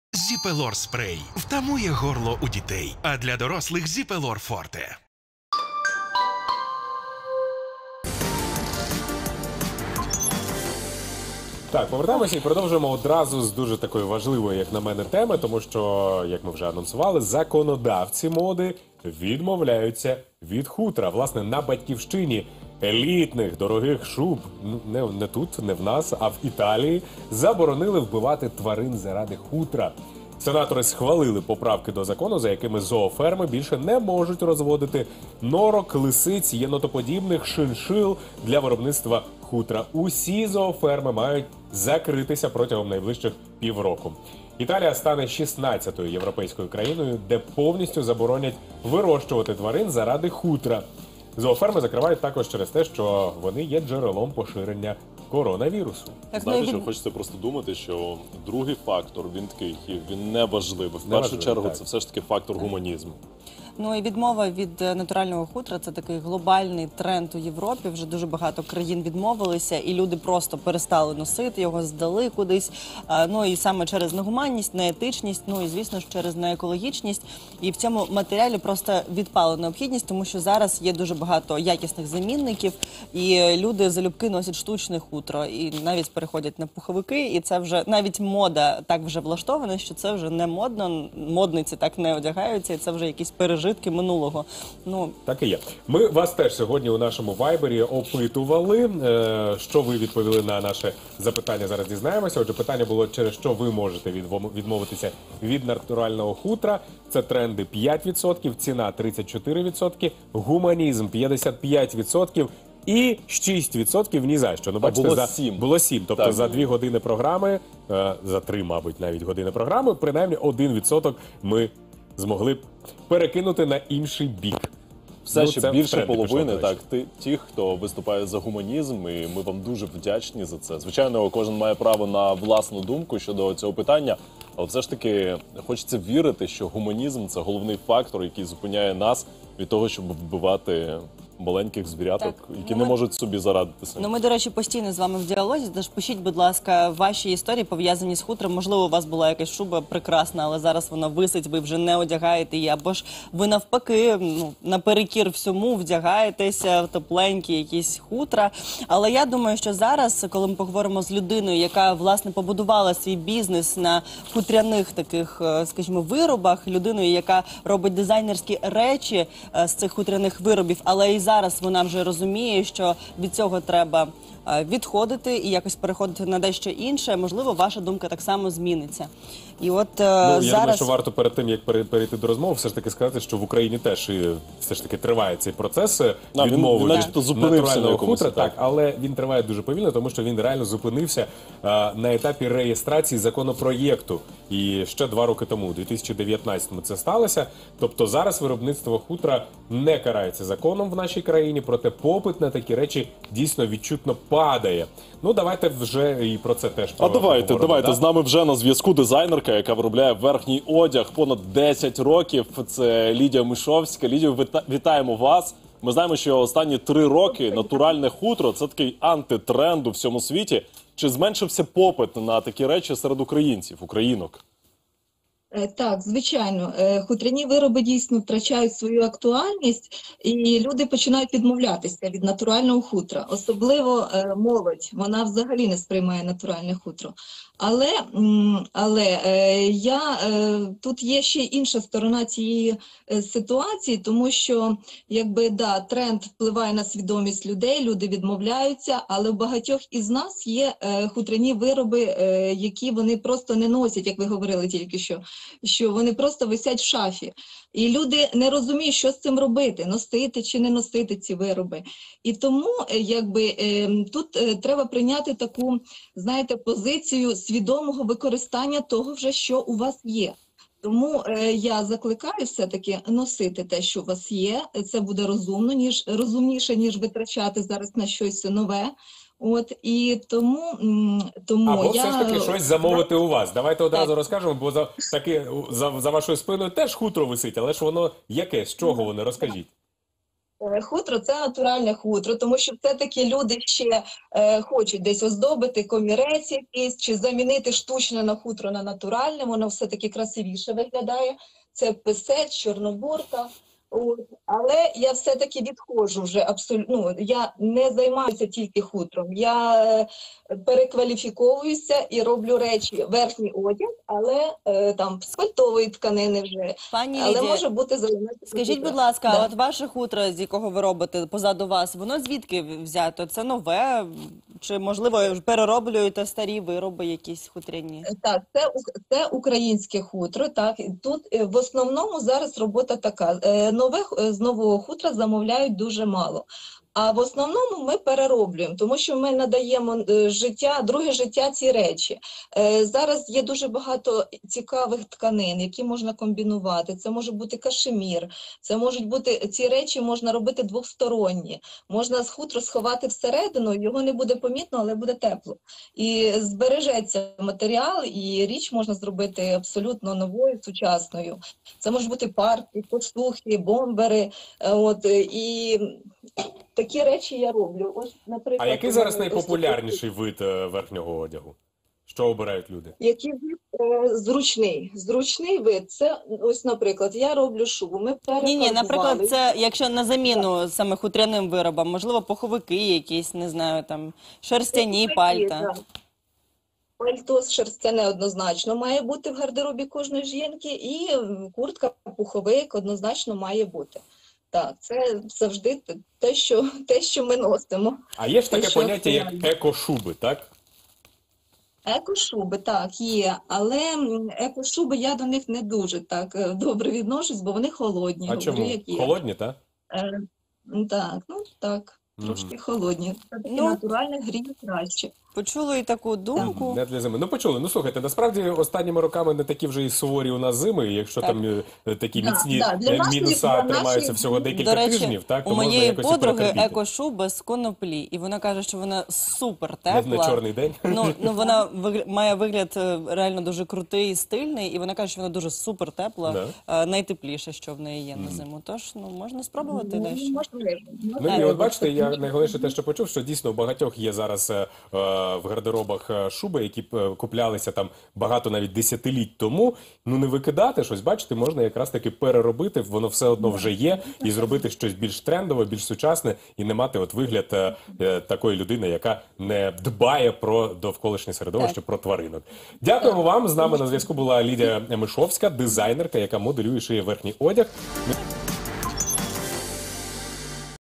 Чи готові українці відмовитися від виробів з натурального хутра – дизайнерка лідія Мишовська
Законодавці моди відмовляються від хутра! На Батьківщині елітних та дорогих шуб, в Італії, заборонили вбивати тварин заради хутра. Сенатори схвалили поправки до закону, за якими зооферми більше не можуть розводити норок, лисиць, єнотоподібних та шиншил для виробництва хутра. Усі зооферми мають закритися протягом півроку. Італія стане 16-ю європейською країною, де повністю заборонять вирощувати тварин заради хутра. В Україні процес відмови від натурального хутра зупинився на етапі реєстрації законопроекту ще в 2019-у році. Тобто зараз виробництво хутра не карається законом. Проте попит на такі речі відчутно падає. Про це зараз нам детальніше розкаже дизайнерка, яка виробляє верхній одяг понад 10 років, Лідія Мишовська.